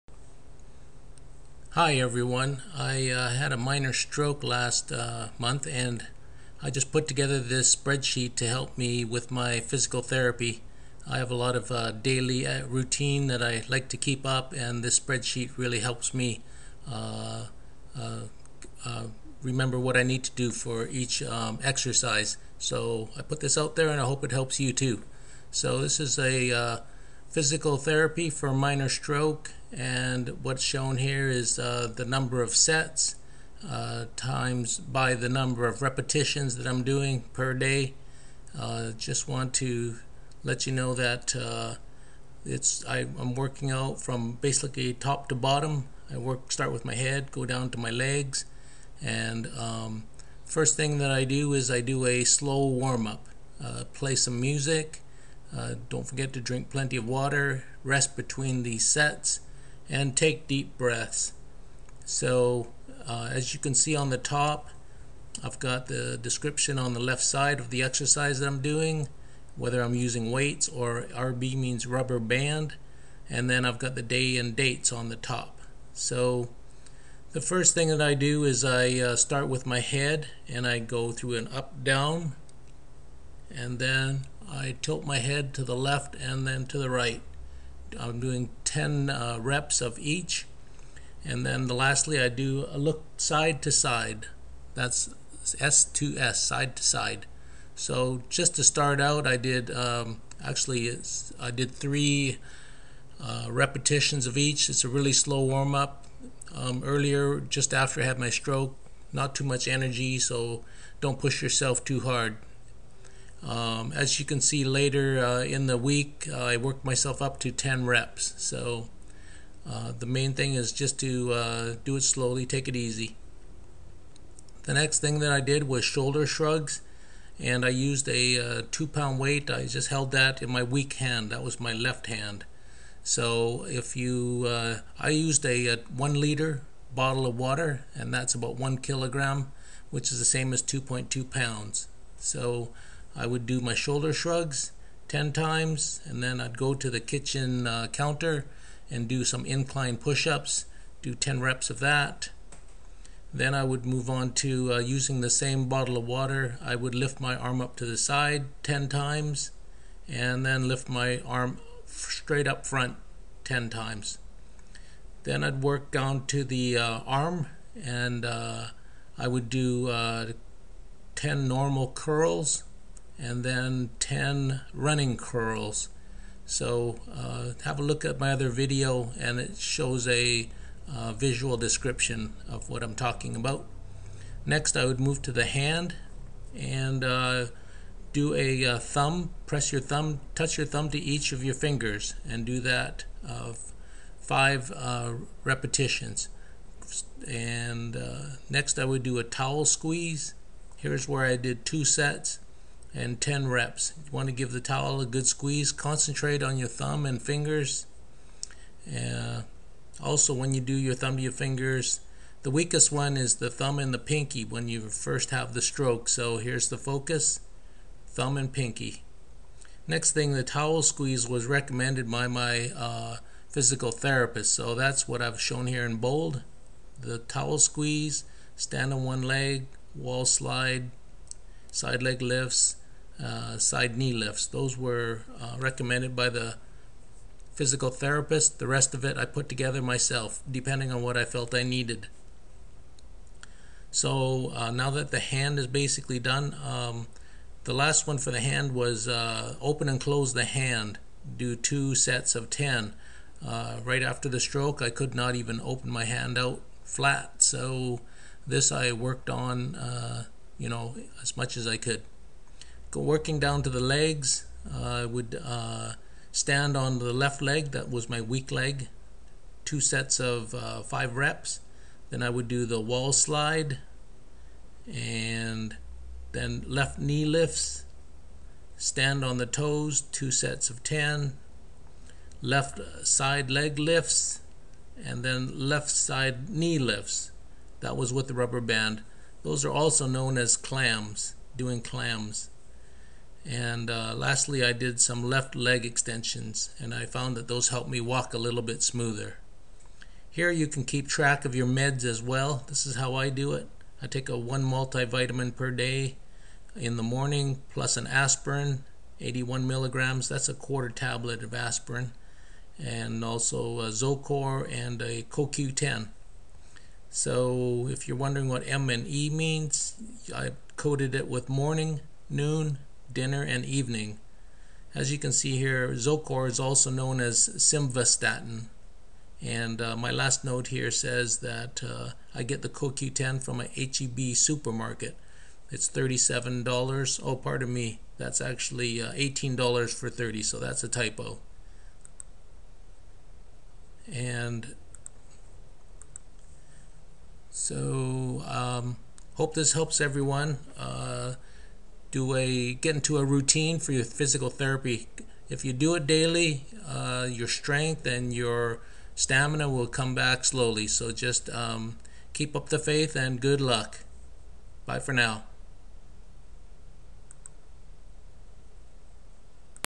Minor Stroke 3 - List of Physical Therapy Exercises (Stroke Recovery)
This is my List of PT Exercises that I used to do my "HomeWork".
I will send a copy.
(Hope this helps you with your Stroke Recovery,